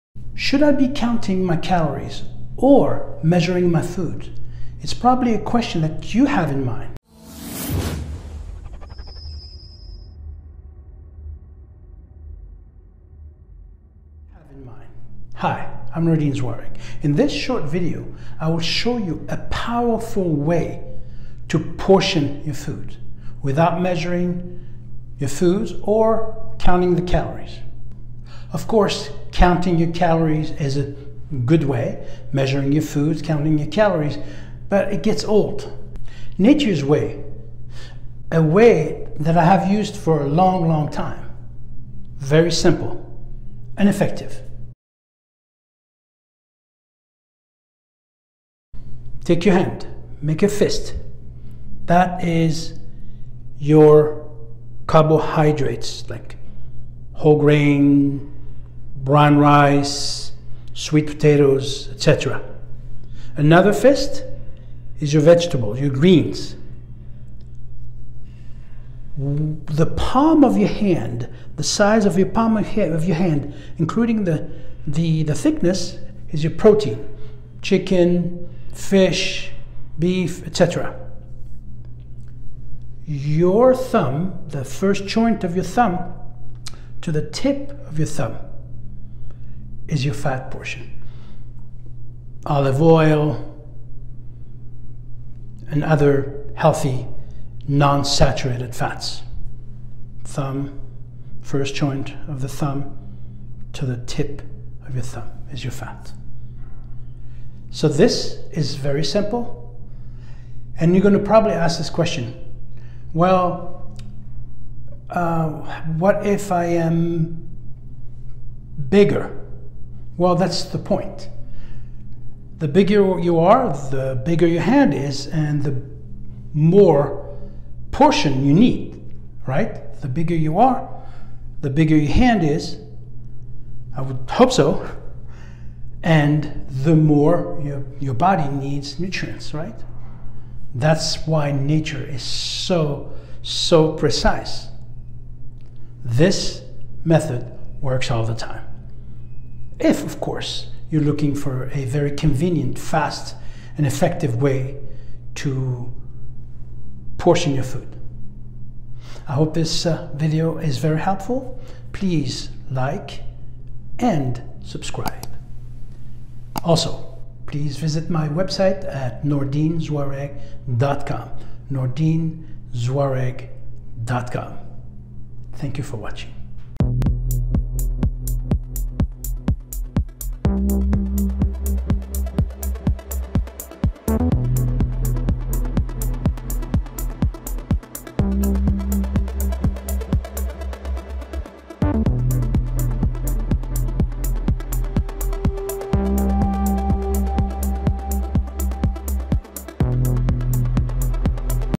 Stop Counting Calories, Do This Instead | Nordine Zouareg
Stop Counting Calories, Do This Instead!

Are you tired of counting calories and measuring your food? Watch former Mr. Universe and bestselling author, Nordine Zouareg share a powerful and effective method to portion your meals.